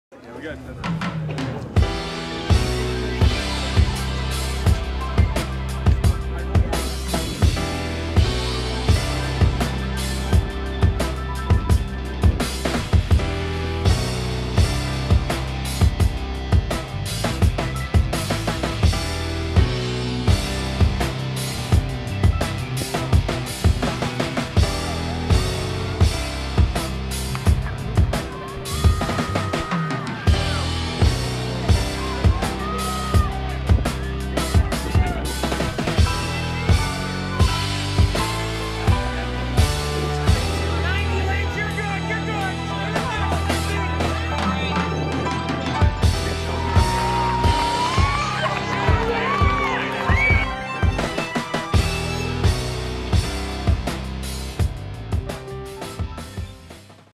Grace Bible Track & Field - GVSU Al Owens Classic
A short glimpse of the GVSU Al Owens Classic.